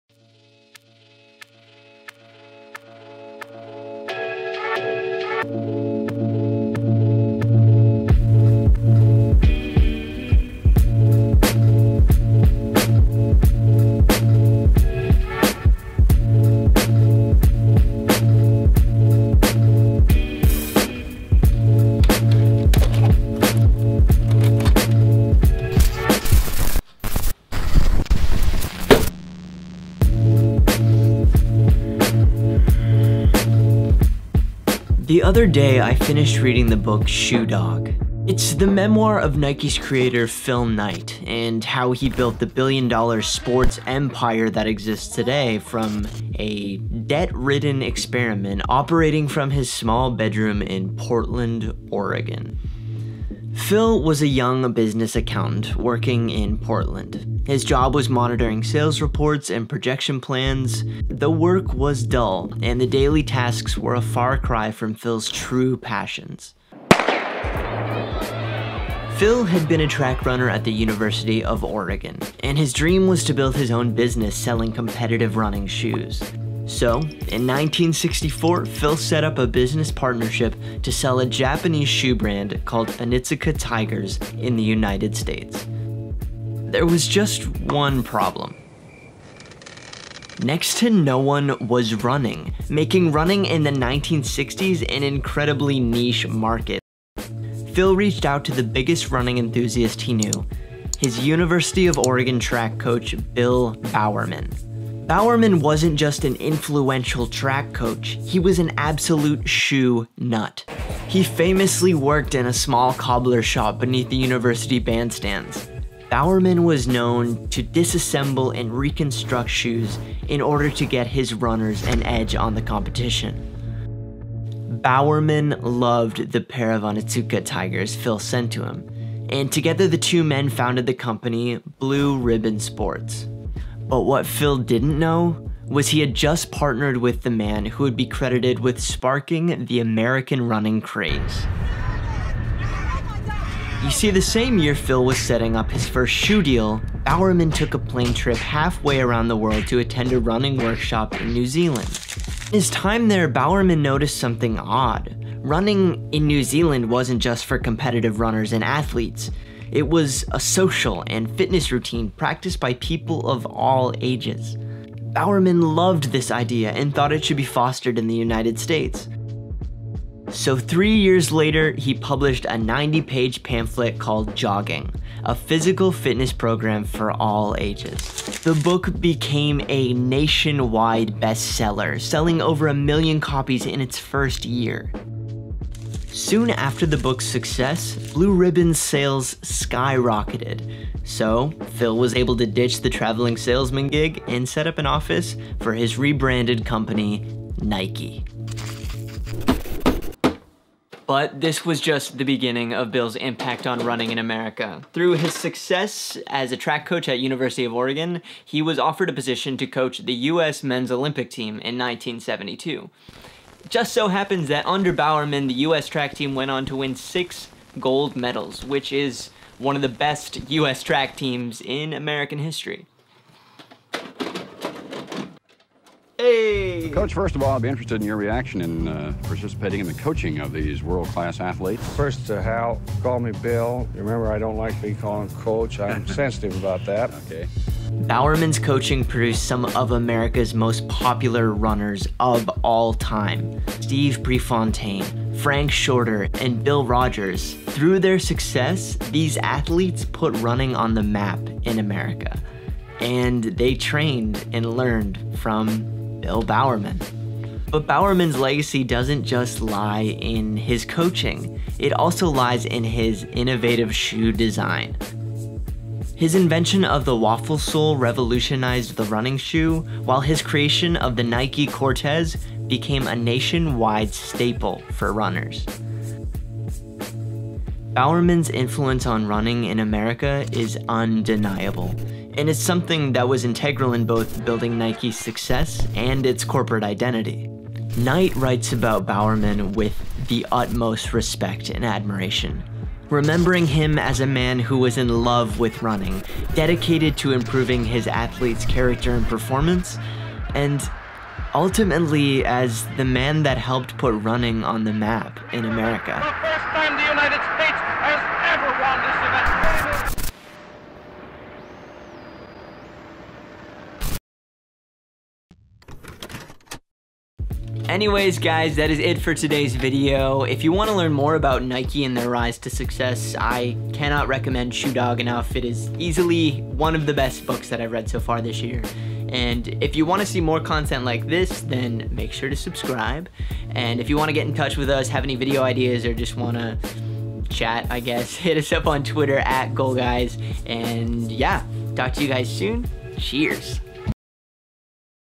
How Nike Made Running Popular in the U.S.A.
Recently finished reading the book Shoe Dog, the memoir of Nike's founder Phil Knight.  Though the most interesting thing I left after reading the books had less to do with Nike's development as a company, but how Nike helped make running popular in America!

Hope you like the video! Make sure to hit us up on twitter or facebook, or in the comments!!